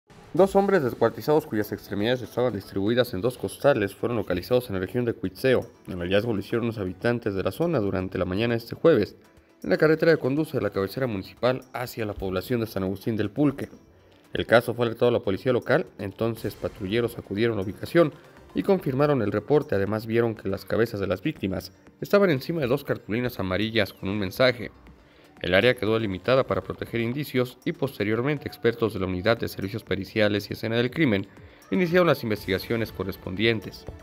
Localizan a 2 hombres descuartizados en el municipio de Cuitzeo
+ Los homicidas les dejaron un mensaje en cartulinas amarillas, cuyo contenido no fue revelado por la Policía.

Cuitzeo, Mich.- Jueves 11 de noviembre de 2021.- Dos hombres descuartizados, cuyas extremidades estaban distribuidas en dos costales, fueron localizados en la región de Cuitzeo, informaron contactos cercanos a las autoridades policiales.

El hallazgo lo hicieron unos habitantes de la zona durante este jueves en la carretera que conduce de la cabecera municipal hacia la población de San Agustín del Pulque.

El caso fue alertado a la Policía local, entonces los patrulleros acudieron a la ubicación y confirmaron el reporte, además, vieron que las cabezas de las víctimas estaban encima de dos cartulinas amarillas con un mensaje, el cual no revelaron los uniformados.

El área quedó delimitada para proteger los posibles indicios, posteriormente arribaron los expertos de la Unidad de Servicios Periciales y Escena del Crimen (USPEC), dependientes de la Fiscalía General del Estado (FGE), quienes iniciaron las investigaciones correspondientes y trasladaron los restos humanos a la morgue.